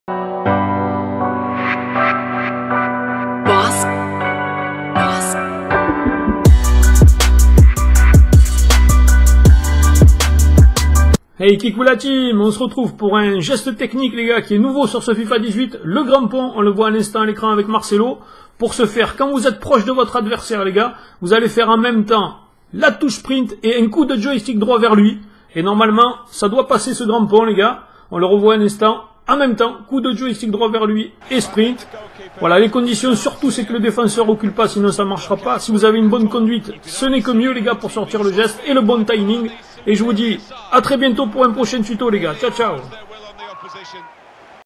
Tuto Skill: le grand pont "the bridge skill"
Coucou la team

on se retrouve pour apprendre un nouveau geste technique "le grand pont"

bon skill à tous